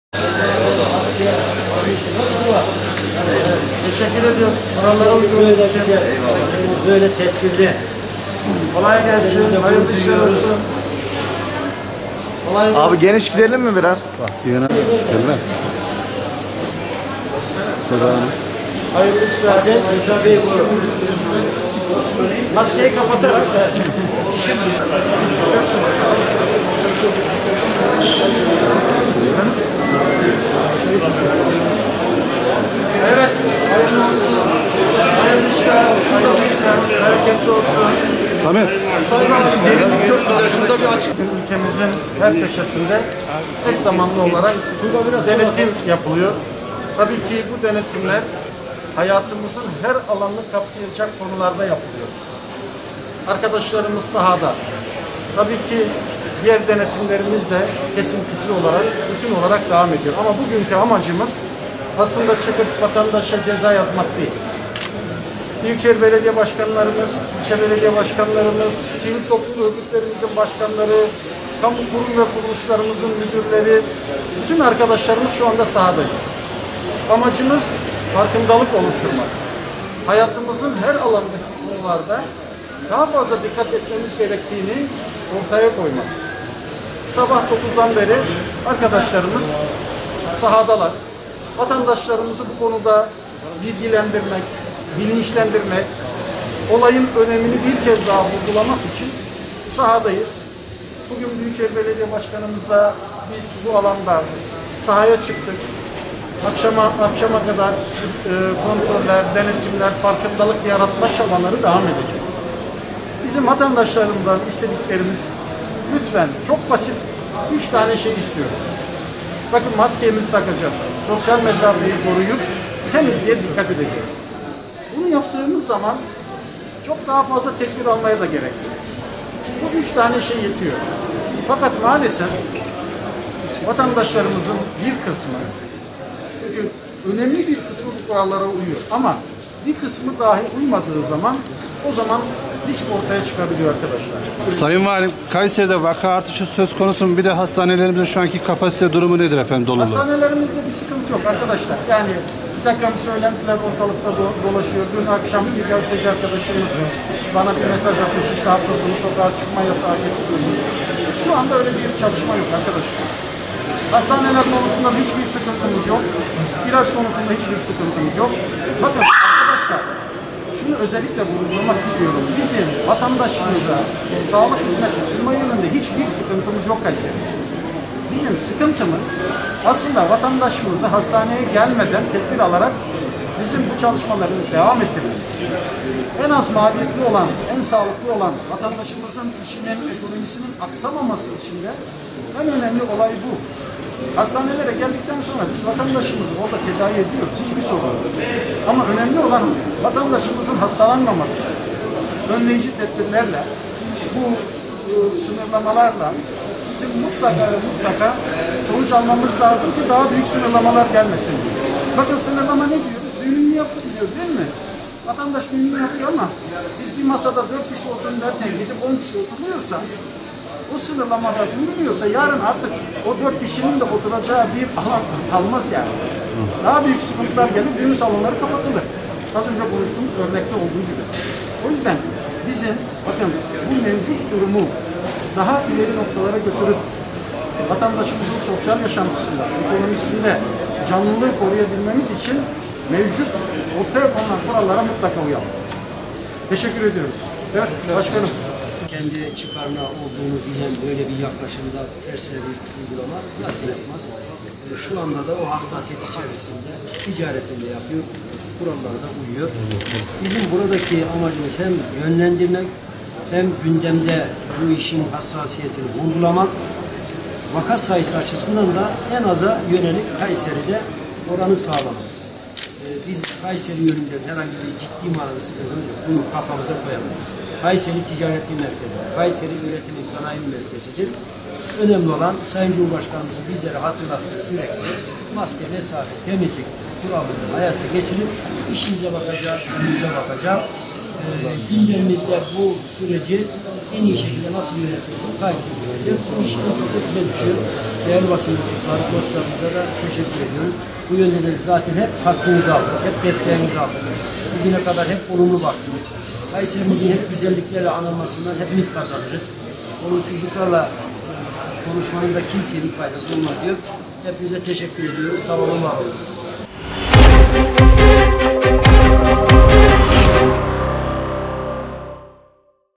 Vali ve Başkan Çarşı Denetimi Yaptı. Darısı Otobüs Denetimine..
Bu gün Türkiye genelinde yoğun olarak uygulanacağı duyurulan denetimler kapsamında Kayseri Valisi Şehmus Günaydın ve Kayseri Büyükşehir Belediye Başkanı Memduh Büyükkılıç, Kazancılar ve Bankalar Caddesinde denetime çıktı. Vali Günaydın, sabah saatlerinde ise Talas'ta bulunan halk otobüsleri hareket amirliğini kontrol ederek güne başladı.  Henüz haber merkezimize gelen, sabah ve akşam saatleri otobüs denetimi ise olmadı.

Türkiye genelinde bugün gerçekleştirilen denetimler kapsamında, Kayseri'de de ekipler sahaya indi. Vali Şehmus Günaydın ve Büyükşehir Belediye Başkanı Memduh Büyükkılıç da farkındalık oluşturmak amacıyla denetimlere katıldı. Kazancılar Çarşısı, 27 Mayıs Caddesi ve Kapalı Çarşı gibi alışverişin yoğun yapıldığı alanlarda denetim yapan Vali Günaydın ve Başkan Büyükkılıç, kurallara uyulması konusunda vatandaşı ve esnafı uyardı.

Vali Günaydın, "81 ilimizde eş zamanlı olarak Büyükşehir Belediye Başkanı'mızın da katılımı ile maske ve sosyal mesafe denetimi yaptık. Yapılan denetimler hayatımızın her alanını kapsayacak alanlarda yapılıyor. Tüm ekiplerimiz sahada denetleme yapıyor. Bugünkü amacımız vatandaşa ceza yazmak değil. Bizim amacımız farkındalık oluşturmak. Vatandaşlarımız maske takmalı ve sosyal mesafe kurallarına uymalıdır. Vatandaşlarımızın bir kısmı kurallara uymadığı zaman risk ortaya çıkıyor. Düğün salonları yetkilileri ile bir araya geleceğiz. Sosyal mesafe ve maske konularında uyarılarda bulunacağız. Hastanelerimizde kapasite sıkıntımız yok. Sokağa çıkma yasağı konuları hakkında bir çalışma yok" dedi.

Başkan Büyükkılıç ise vatandaşın hassasiyet içinde sosyal mesafe ve maske kurallarına uyduğunu, belediye olarak ilgili yetkililerin denetimleri aralıksız olarak sürdürdüğünü kaydetti.